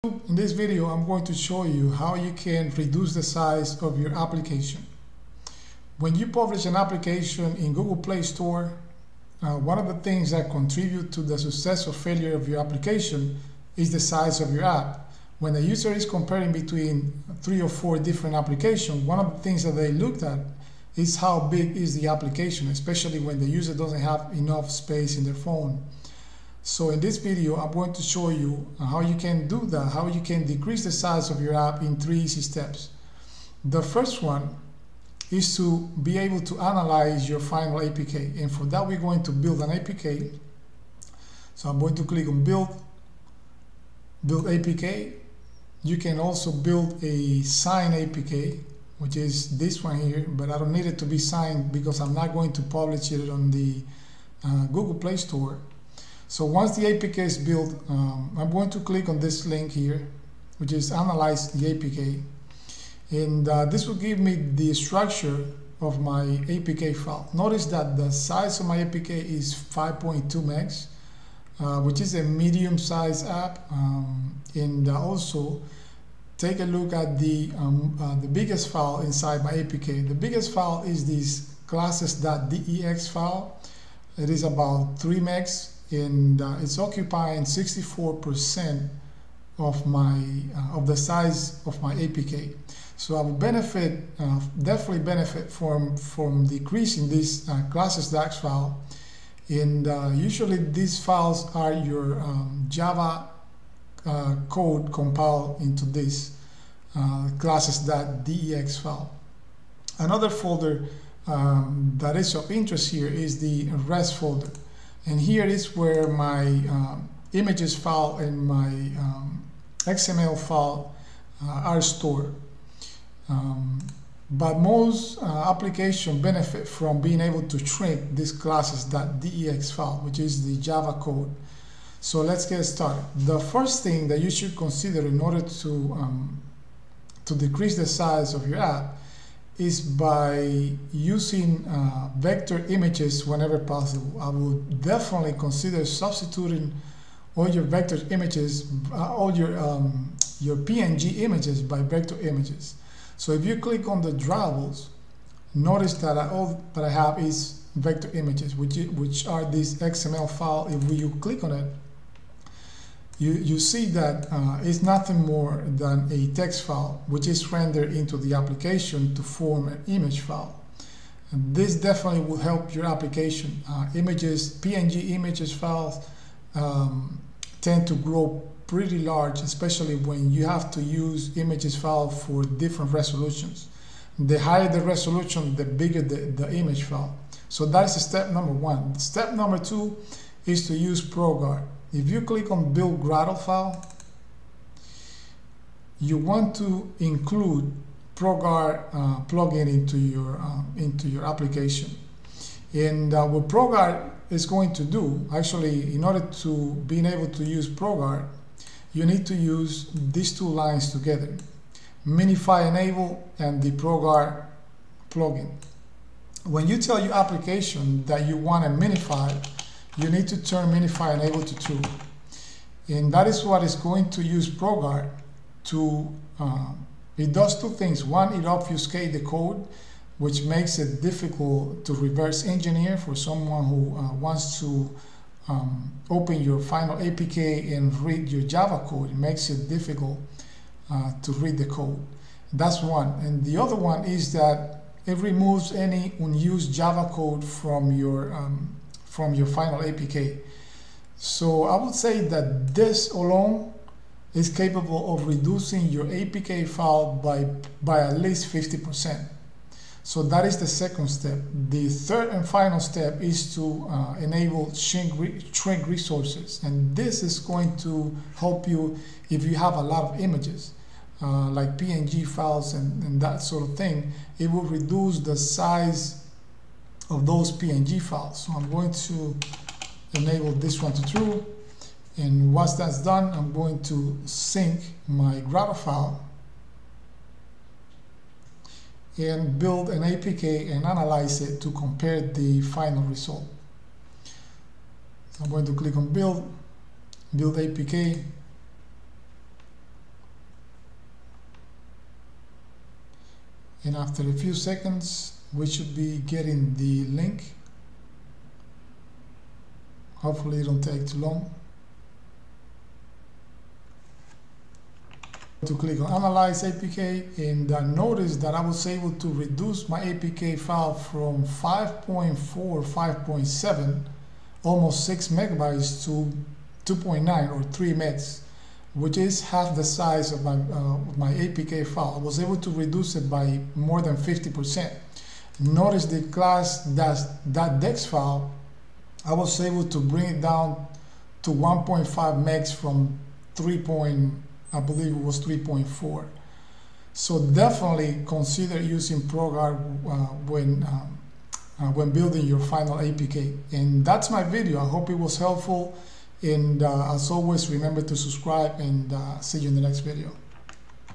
How to Reduce The Size of Android APK Application in 3 Steps
In this video I'm going to show you how to reduce the size of your android apk file in Android Studio.
Content
Build and Analyze APK
Use Vector Images
Use Progard
Use Shrinkresources